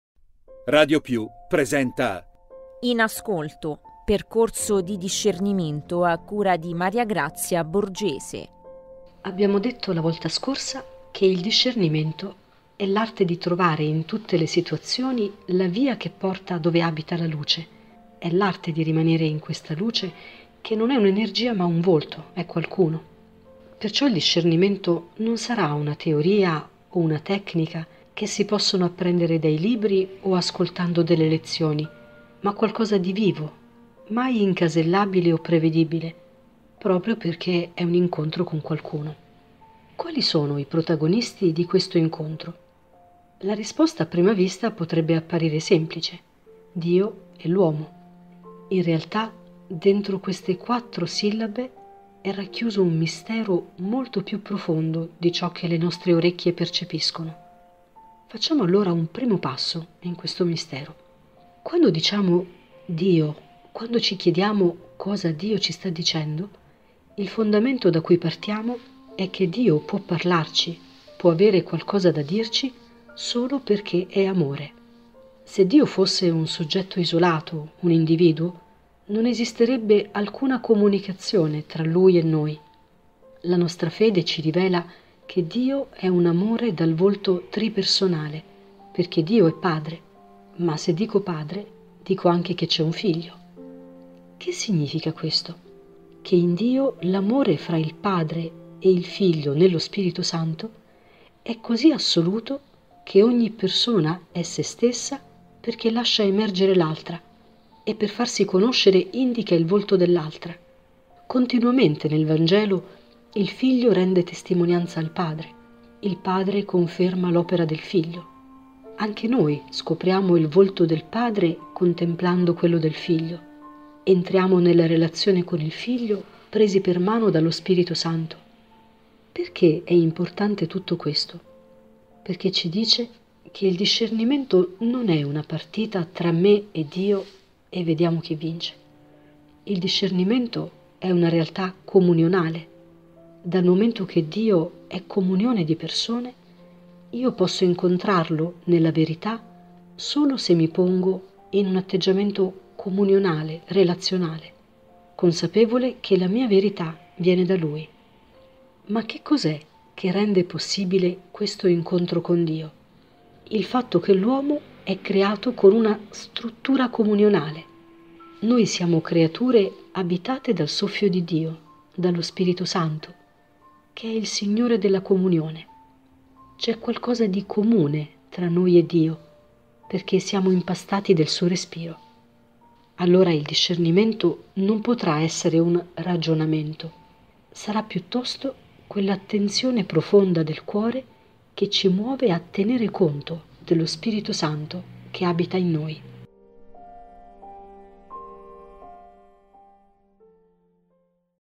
In ascolto - I protagonisti del discernimento spirituale
Il discernimento non potrà essere un ragionamento. Sarà piuttosto quell’attenzione profonda del cuore che ci muove a tenere conto dello Spirito Santo che abita in noi. La riflessione della professoressa Maria Grazia Borgese